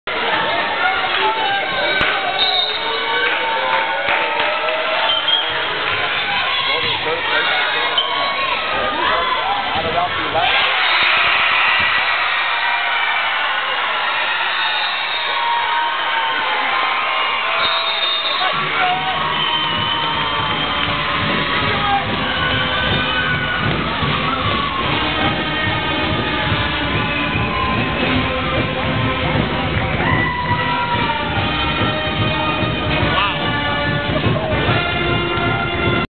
Roelike's kill gives Jaguars game 1 vs. Nevis 11-06-2010
Game one score: 25-22 in favor of the Jaguars in the Section 5A championship match on Saturday, November 6, 2010 at Pierz. Archive video by the Bonanza Valley Voice newspaper in Brooten, MN.